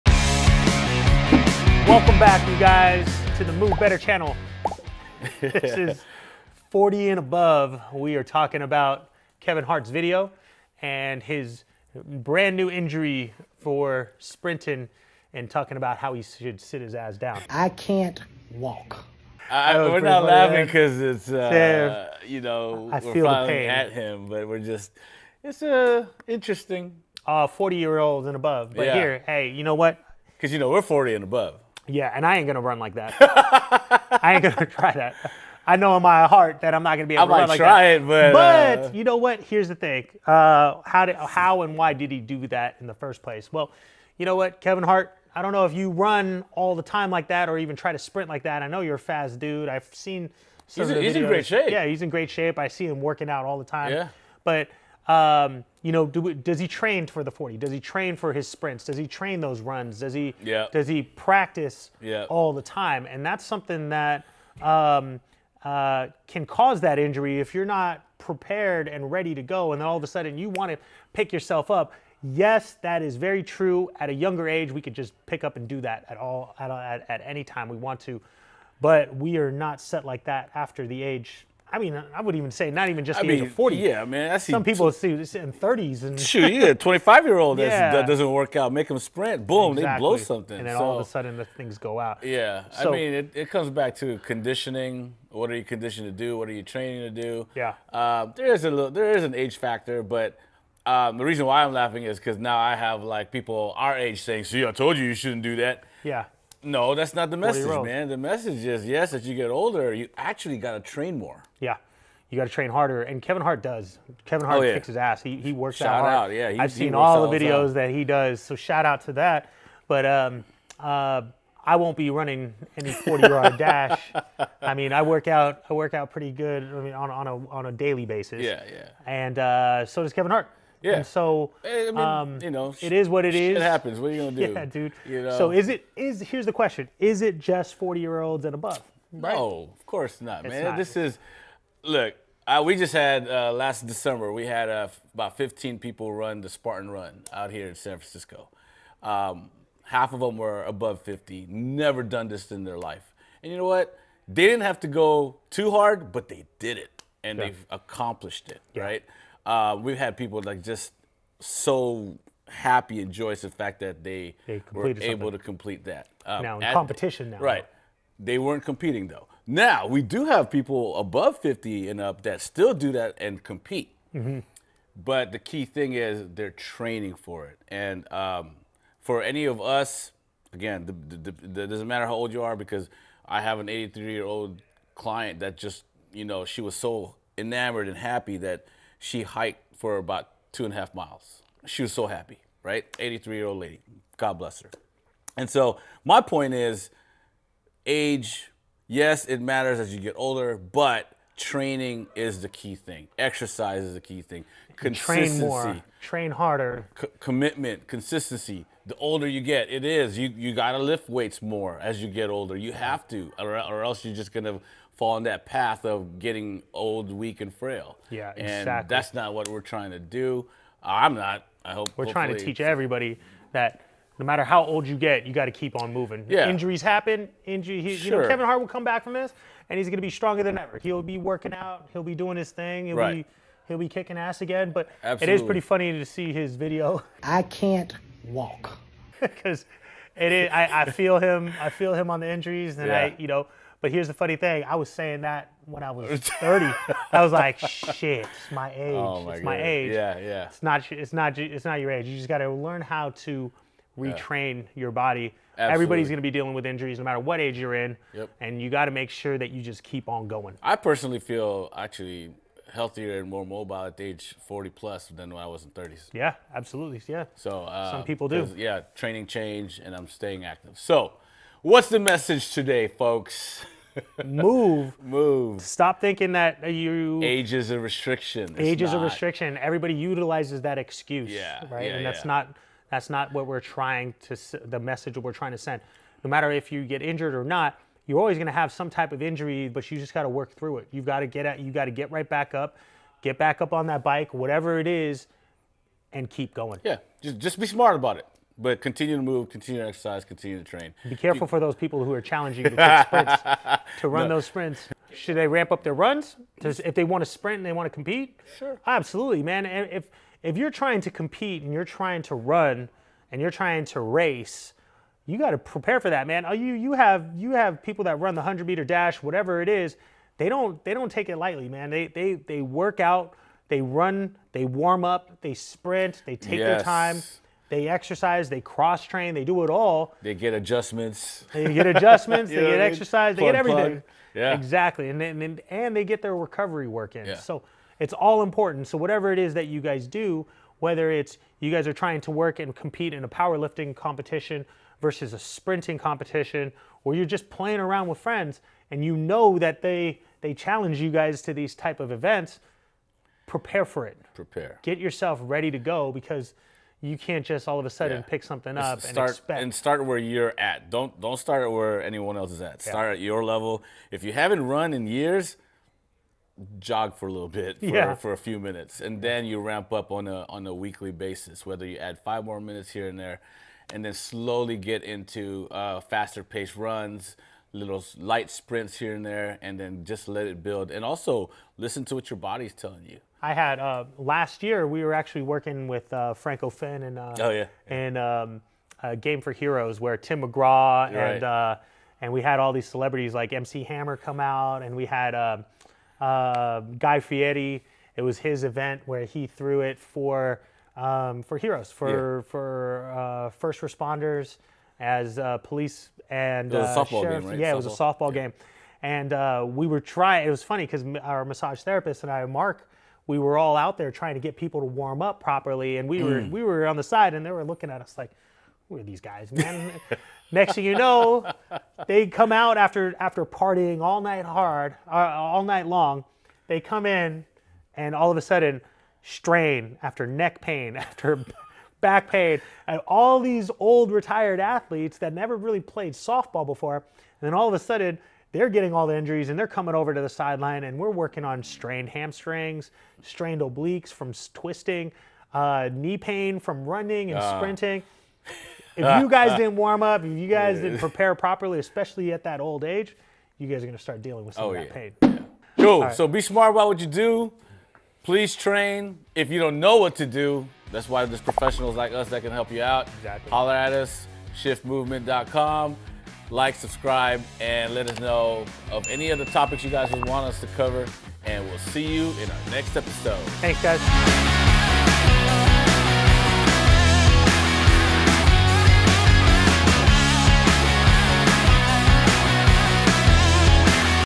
40 Year Olds, Don't Stop Moving!
Are you over 40 and hesitant about staying active? Dr. Joe and John Penacerrada from the 'Move Better' channel are here to change your perspective with "40 Year Olds, Don't Stop Moving!" Dive deep into the world of gerontology and discover the undeniable importance of movement, especially as we age.

In this video, we discuss the recent sprinting injury of Kevin Hart and what it implies about age and physical activity. Learn about the significance of conditioning and training, especially as we grow older. We debunk myths surrounding age and movement, emphasizing that age shouldn't deter one from staying active. It's not just about age; it's about preparation, conditioning, and understanding your body's needs.

Hear inspiring stories of individuals over 50 achieving remarkable physical feats and understand why age is just a number. Whether you're dealing with injuries or just want to improve your overall health, this video is your comprehensive guide to staying active and moving better at any age.

Your journey to a healthier, more active life starts here.